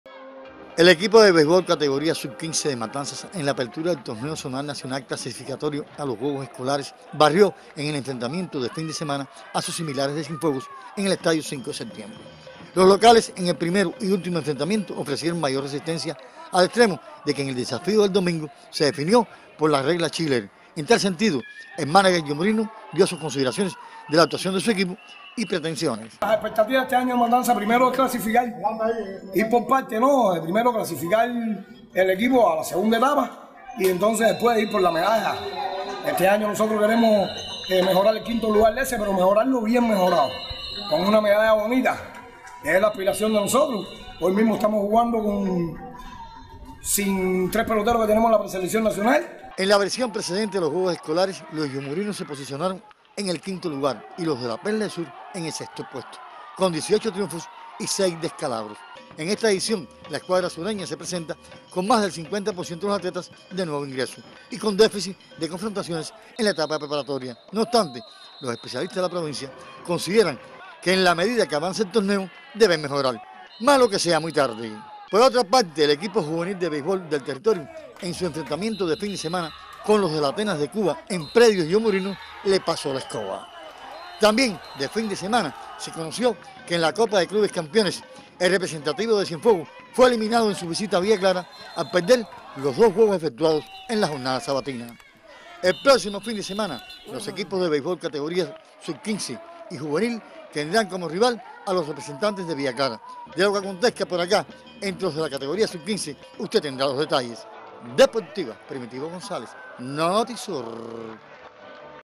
Inició Torneo Zonal de Béisbol
El Torneo Zonal de Béisbol, comenzó este fin de semana en el país. Veamos lo acontecido en el estadio 5 de Septiembre, donde se enfrentaron las novenas de Cienfuegos y Matanzas.